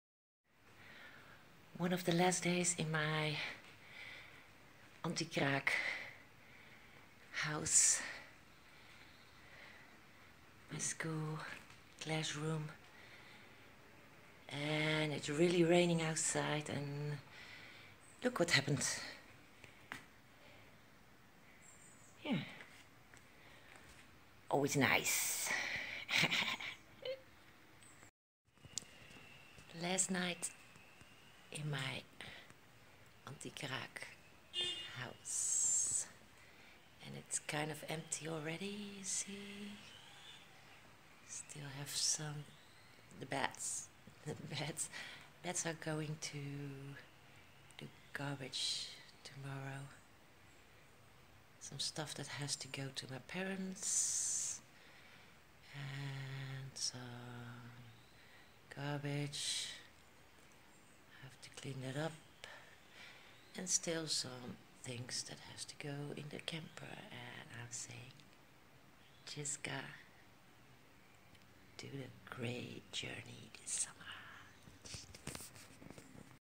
01 Last day in my house
The last day in my anti squad / anti kraak house